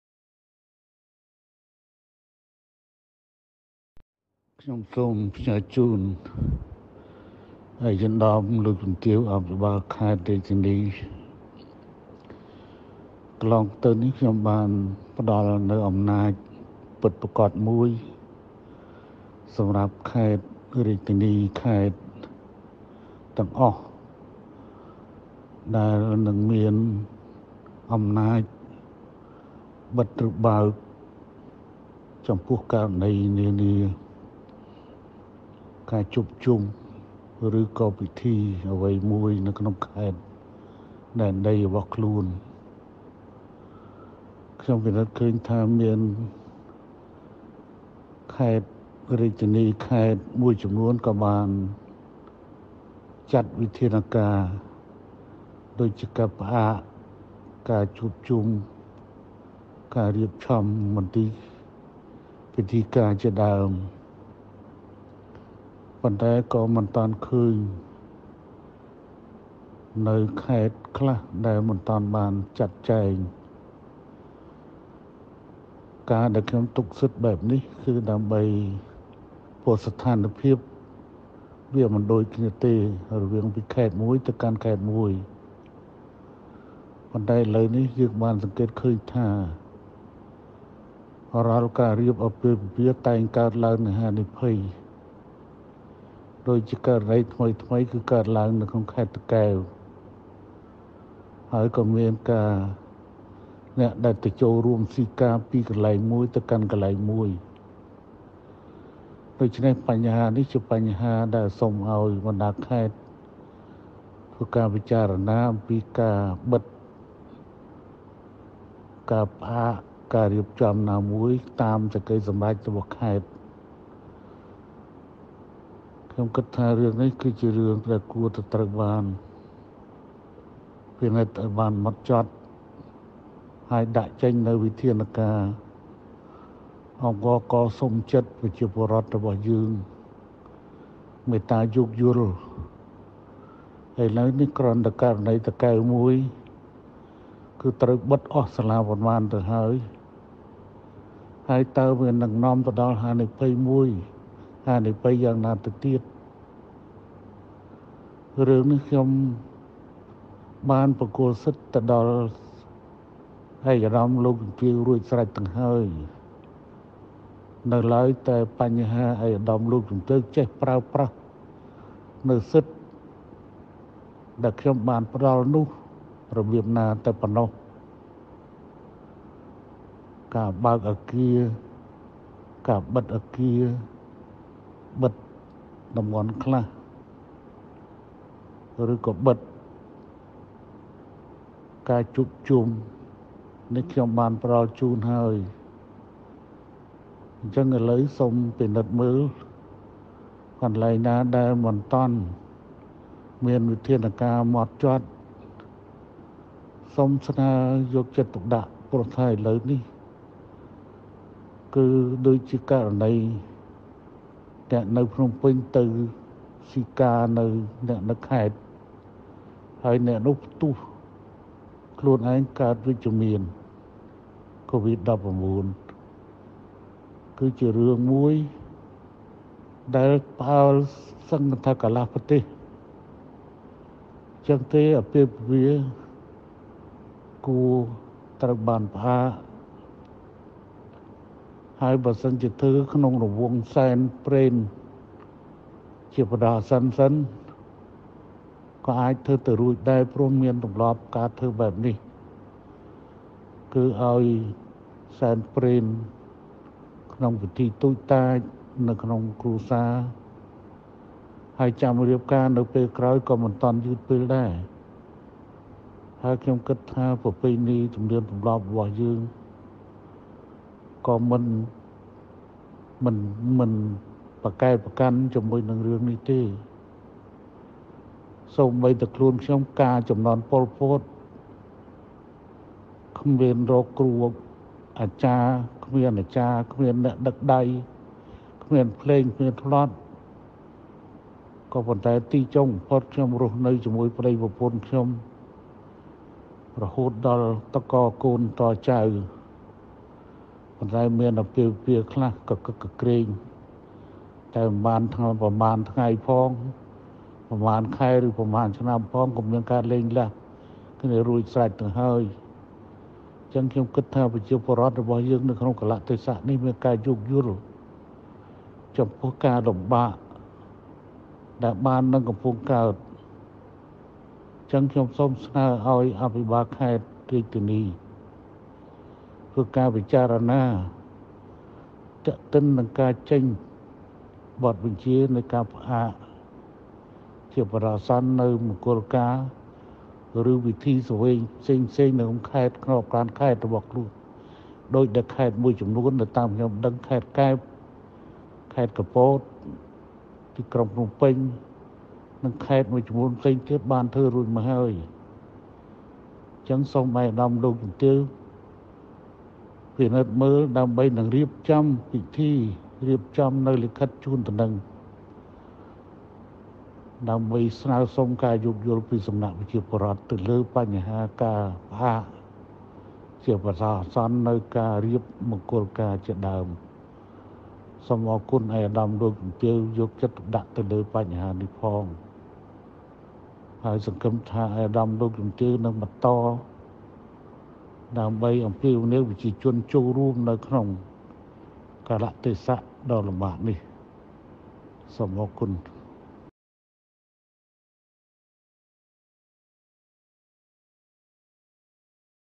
លោកនាយករដ្ឋមន្រ្តី ណែនាំ​ឱ្យ​អភិបាល​ខេត្ត​ទាំង​អស់​ បិទការរៀបការ និង ពិធីជួបជុំនានា នៅទូទាំងប្រទេស
សម្ដេចតេជោណែនាំ​ឱ្យ​អភិបាល​ខេត្ត​ទាំង​អស់​ផ្អាក​ពិធីមង្គលការ និងពិធីជួបជុំនានា នៅទូទាំងប្រទេស។

នៅក្នុង​សារជាសំឡេង​ សម្តេច​តេជោ​លេីកឡេីងថា​ សូមពលរដ្ឋ​យោគយល់​ ពីព្រោះគ្រាន់តែក្នុងករណី​នៅខេត្ត​តាកែវ​មួយ​ធ្វេីអោយមានហានិភយ​ច្រើន​ណាស់​ទៅហេីយ​   ដូច្នេះ​សូមអោយ​អភិបាល​ខេត្ត​ទាំង​អស់​ ចេញ​លិខិត​ផ្អាកអាពាហ៍ពិពាហ៍​ និង​ពិធី​ជួបជុំ​នានា​នៅក្នុងដែនដីរបស់ខ្លួន​។
ថ្ងៃទី១៥ ខែ មីនា ឆ្នាំ ២០២១